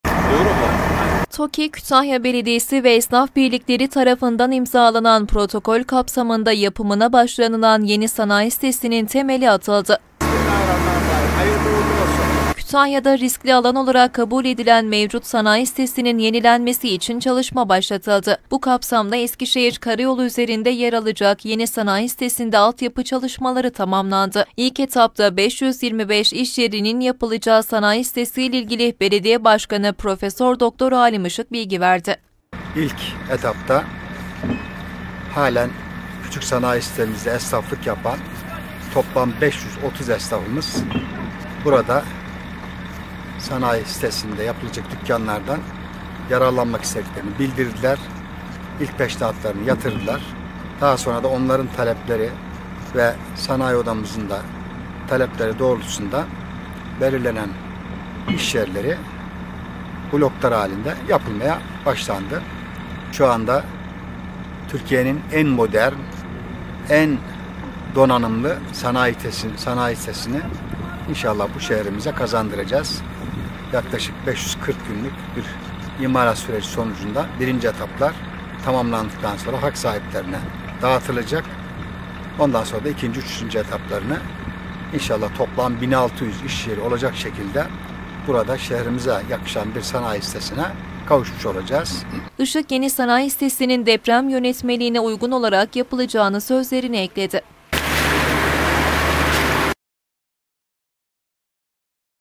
YENİ SANAYİ SİTESİ'NİN TEMELİ ATILDI
Sanayi sitesinin ilk etabında 525 iş yeri olacak